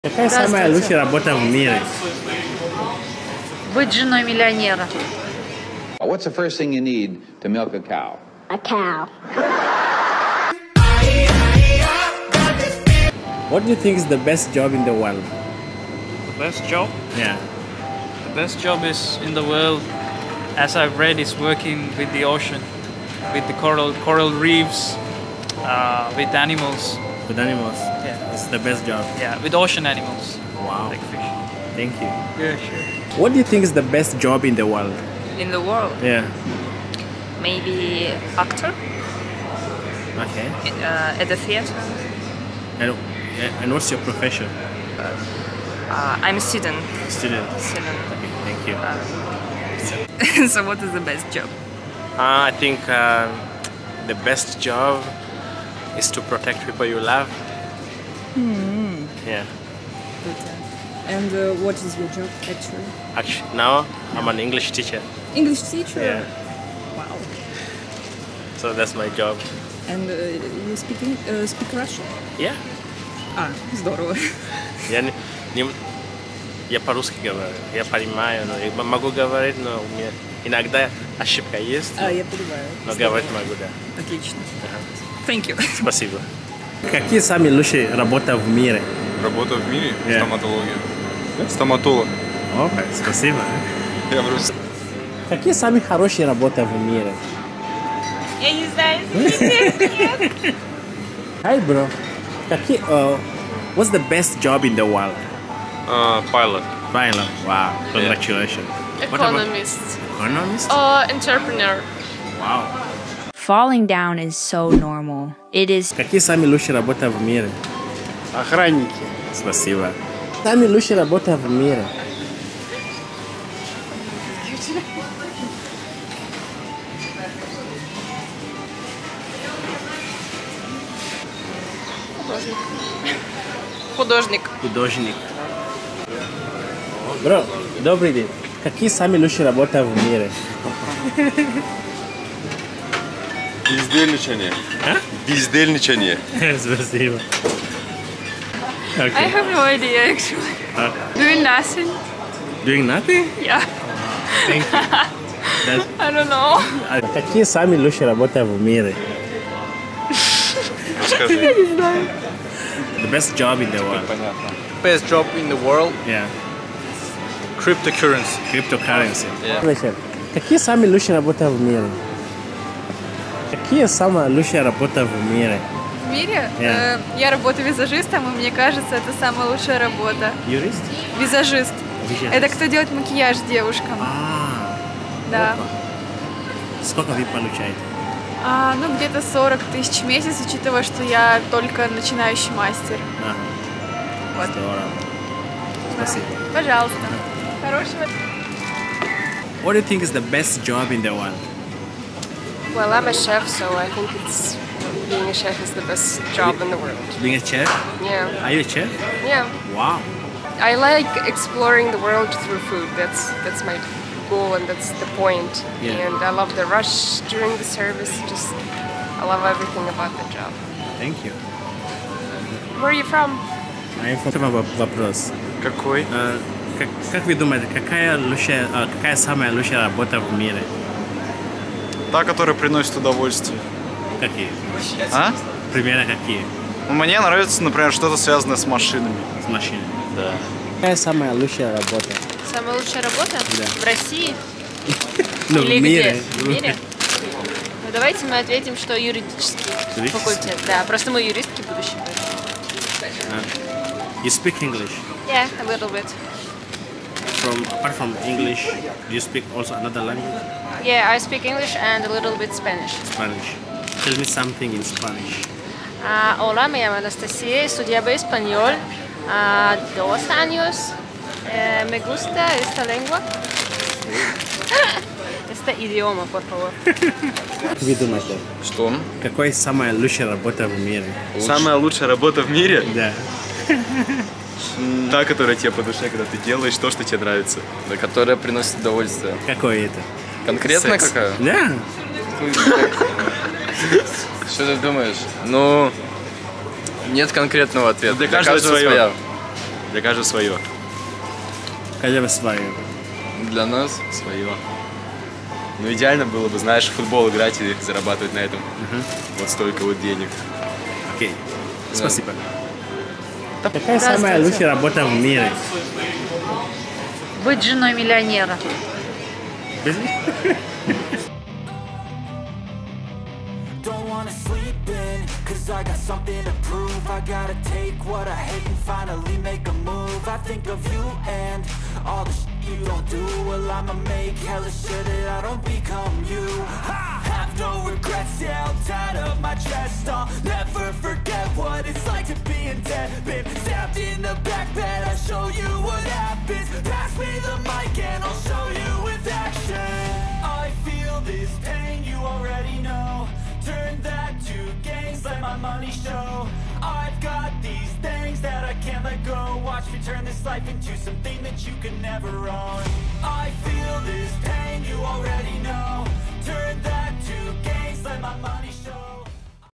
What's the Best Job in The World -2023. Public Opinion - Moscow Russia
Asking people what they think is the best job in the world. What's your opinion?